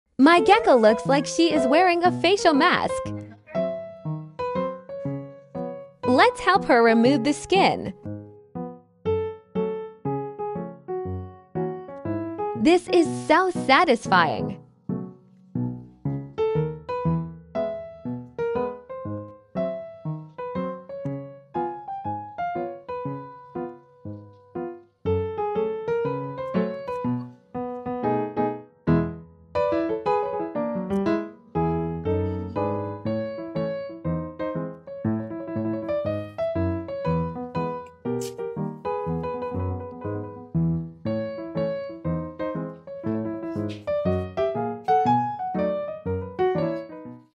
Helping my leopard gecko peel off her skin ❤️
If you like our video please share , give us a huge thumbs up and

⭐️⭐️ Cutie Wonder World Other Social Media Accounts ⭐️⭐️